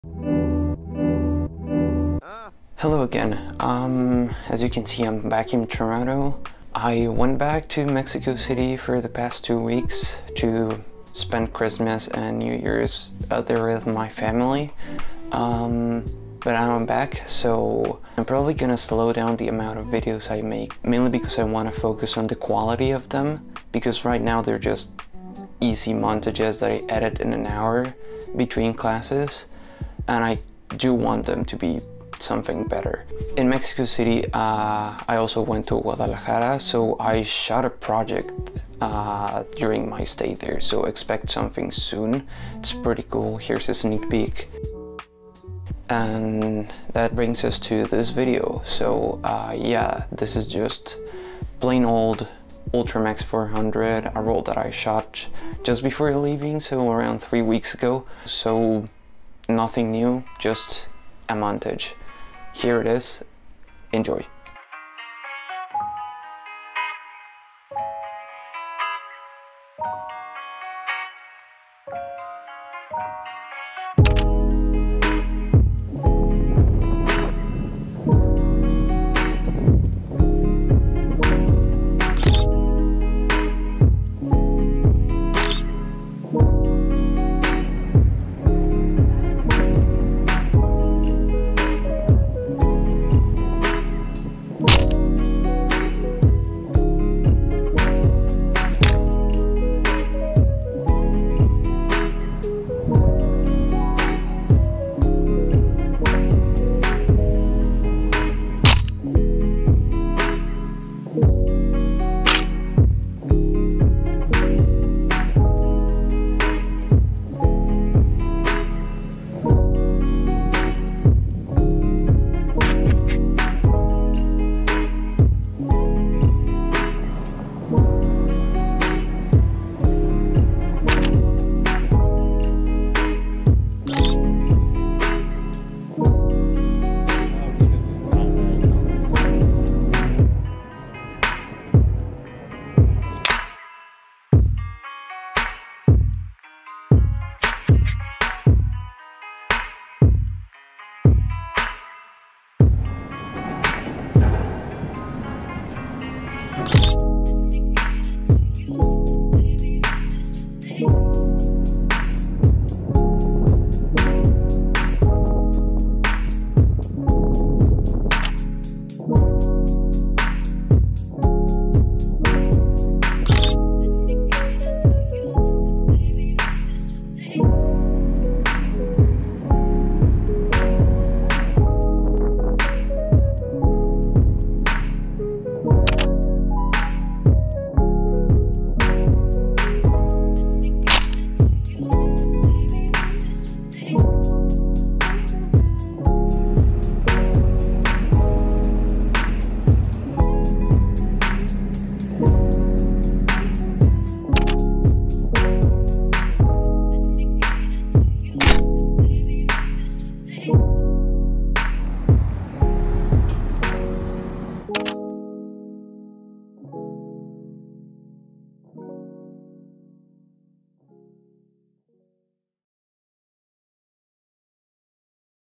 Winter Film Photography | Kodak Ultramax 400 during December
Here's a little video from a roll of Kodak Ultramax 400 I shot around 3 weeks ago.